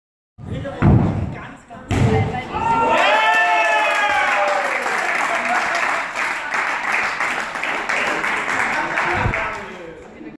Floor - Double Frontflip / Doppelsalto vorwärts
Daniel Gansjuk mit einem Doppelsalto vorwärts am Boden